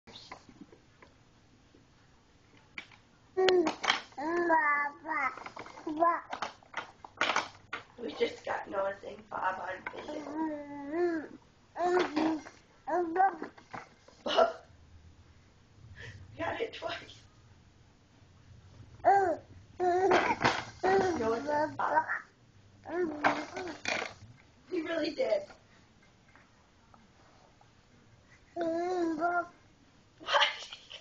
Noah's First Word
Yup, his first word was actually B-O-B, Bob...does that mean he has inherited a pontoon and 4-wheeler Uncle Bob?!?!